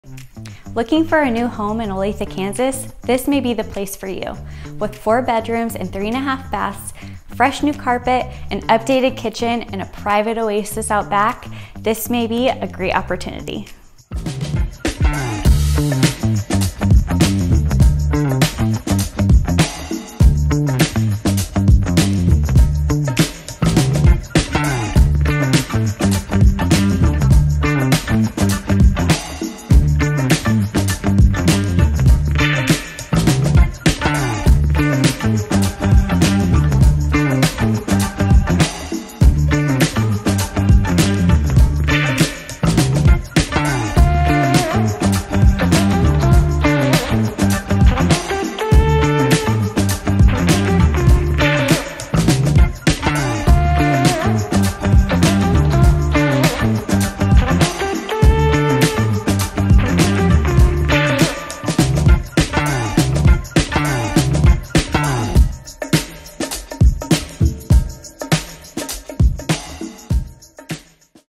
For Sale in Olathe KS
Welcome to 15643 S Blackfoot St in Olathe, an excellent location just steps to the park and elementary school. This home sits on a quiet lot that backs to green space and features an updated kitchen, new carpet, and gorgeous finishes. The finished basement provides an additional living area for your pleasure. Come in and check it out.

MLS #2505710
Courtney Hatch / REAL Broker, LLC